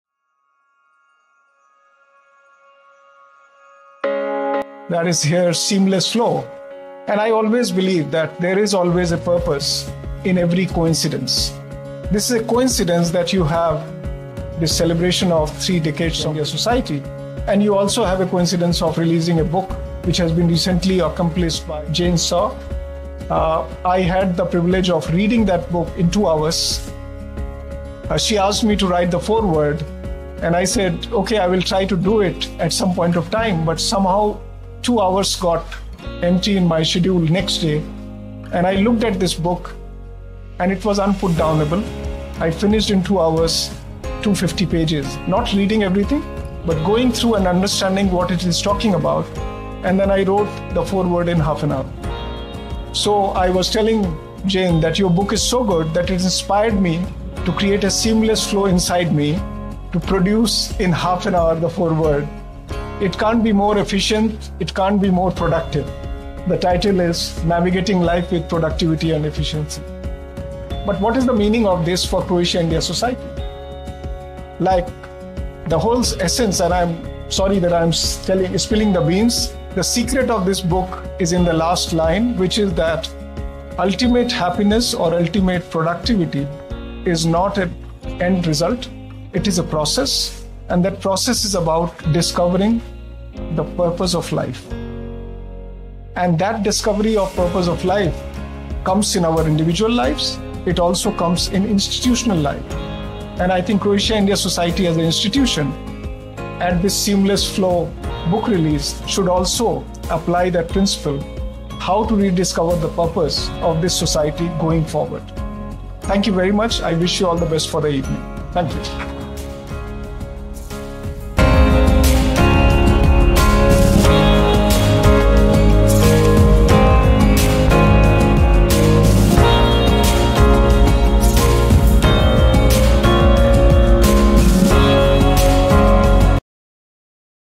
The Launch Event in Zagreb for the book Seamless Flow
On a remarkable day in Zagreb, Croatia, the launch of 'Seamless Flow - Navigating Life with Purpose & Efficiency' took place. The event was attended by distinguished dignitaries, including the Ambassador of India to Croatia, who delivered an inspiring speech. This gathering celebrated not just a book, but the collective pursuit of a life driven by purpose and efficiency.